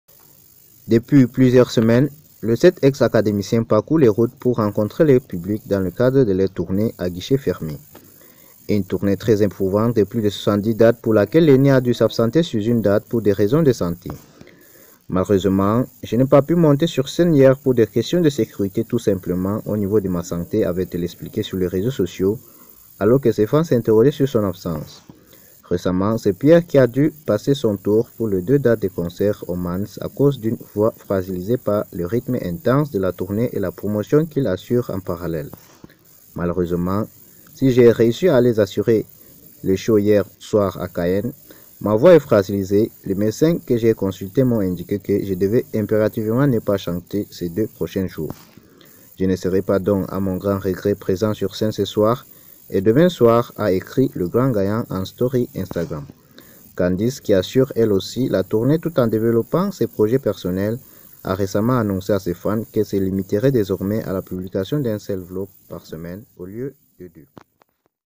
Lénie (Star Ac) révèle avoir frôlé la mort avec Pierre Garnier : «J'étais en réanimation à cause...»
Lénie (Star Academy) révèle avoir frôlé la mort avec Pierre Garnier(Star ac), «J'étais en réanimation à cause de la...»."Je suis toujours vivante", Lénie (Star Academy) rassure ses fans sur son état de santé. Depuis plusieurs semaines, les sept ex-académiciens parcourent les routes, pour rencontrer leur public dans le cadre de leur tournée à guichets fermés. Une tournée très éprouvante de plus de 70 dates, pour laquelle Lénie a dû s'absenter sur une date, pour raisons de santé. "Malheureusement, je n'ai pas pu monter sur scène hier pour des questions de sécurité tout simplement, au niveau de ma santé", avait-elle expliqué sur les réseaux sociaux, alors que ses fans s'interrogeaient sur son absence. Récemment, c'est Pierre qui a dû passer son tour pour les deux dates de concert au Mans, à cause d'une voix fragilisée par le rythme intense de la tournée et la promotion qu'il assure en parallèle.